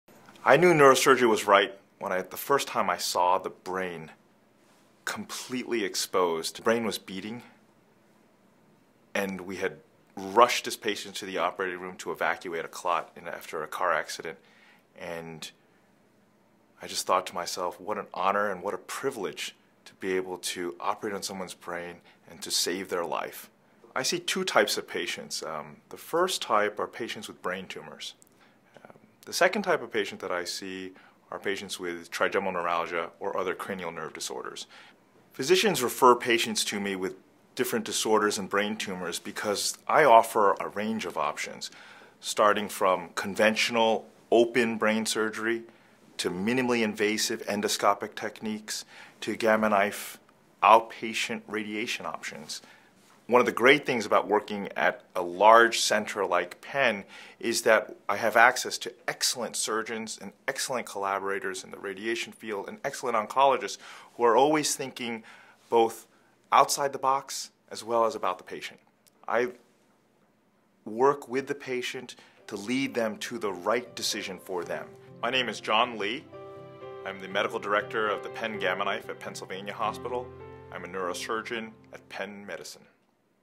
John Y.K. Lee, MD - Neurosurgeon, Penn Medicine
Watch an interview with John Y.K. Lee, MD, a neurosurgeon at Penn Medicine.

"I knew neurosurgery was right when I --the first time I saw the brain completely exposed. The brain was beating. And we had rushed this patient to the operating room to evacuate a clot after a car accident. And I just thought to myself, what an honor and what a privilege to be able to operate on someone's brain and to save their life."

"I see two types of patients. The first type are patients with brain tumors. The second type of patient that I see are patients with trigeminal neuralgia or other cranial nerve disorders. Physicians refer patients to me with different disorders and brain tumors because I offer a range of options starting from conventional open brain surgery to minimally invasive endoscopic techniques, to Gamma Knife outpatient radiation options."

"One of the great things about working at a large center like Penn is that I have access to excellent surgeons and excellent collaborators in the radiation field, and excellent oncologists who are always thinking both outside the box as well as about the patient. I work with the patient to lead them to the right decision for them."

"My name is John Lee, I'm the Medical Director of the Penn Gamma Knife at Pennsylvania Hospital. I'm a neurosurgeon at Penn Medicine."